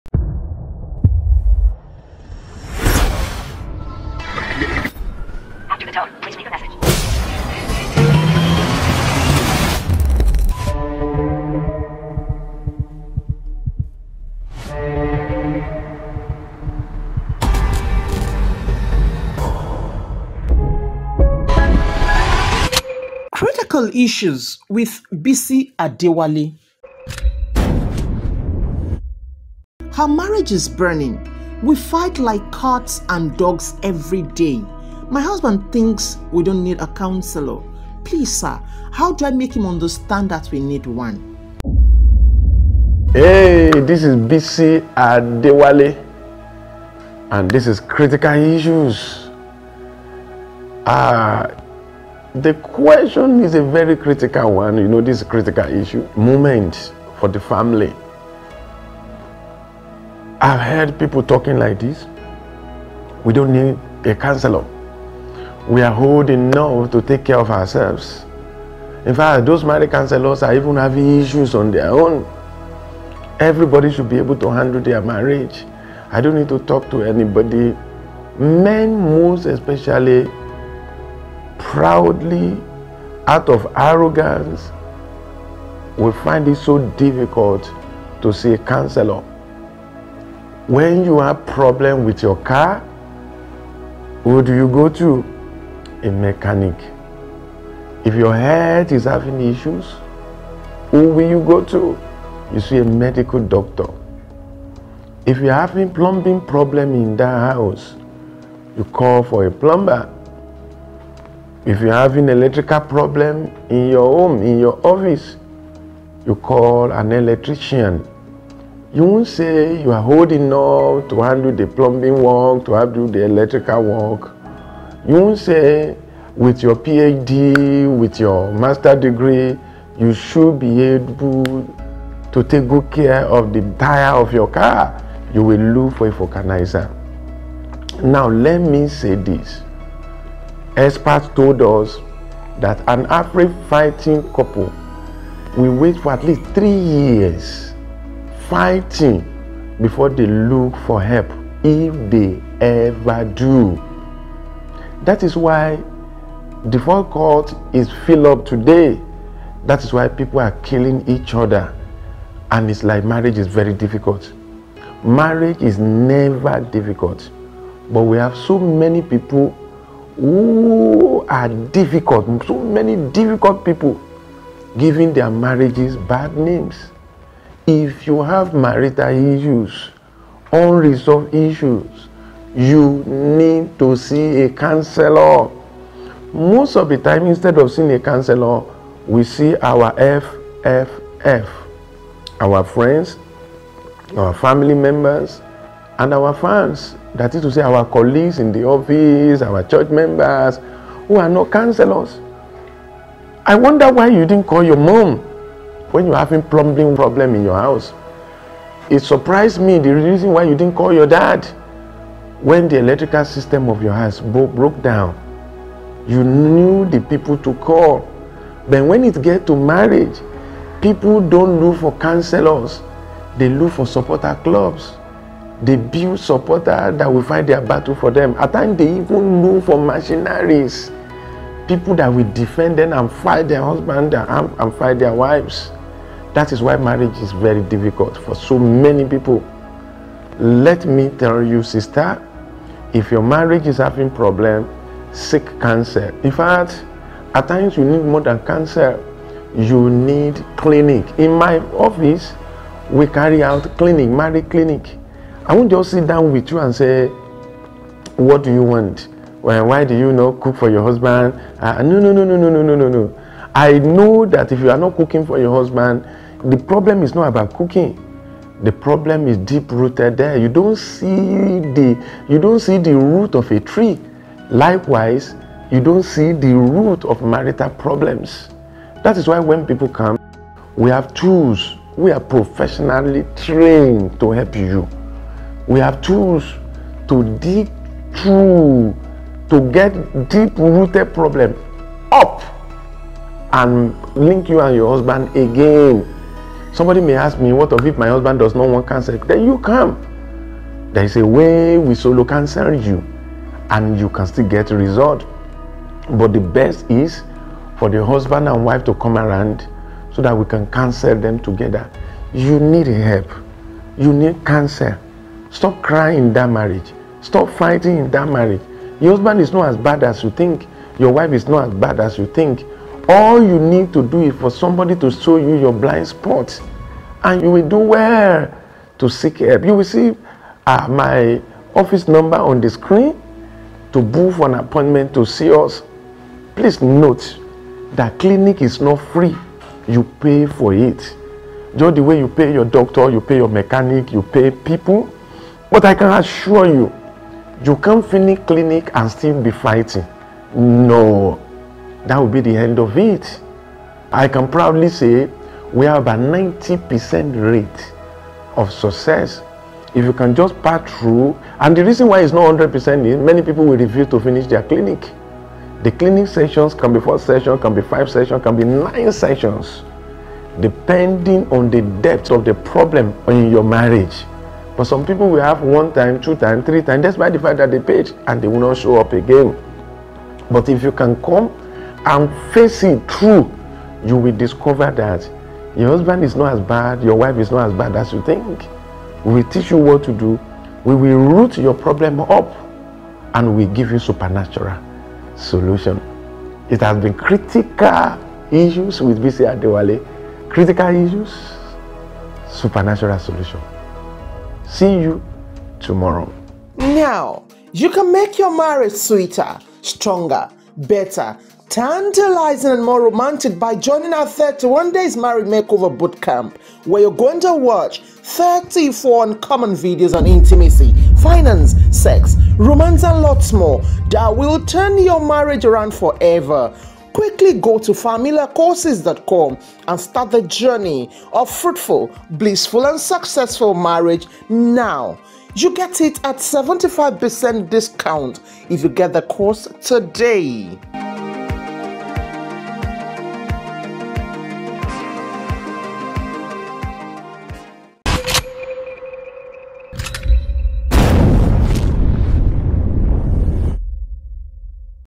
PLEASE HELP!!! MY MARRIAGE IS BURNING | CRITICAL ISSUES
I'm sorry to hear that you're going through a difficult time in your marriage. Here are a few steps you can take to address the issues you're facing BY BISI ADEWALE

©Follow Me Social Media For Anything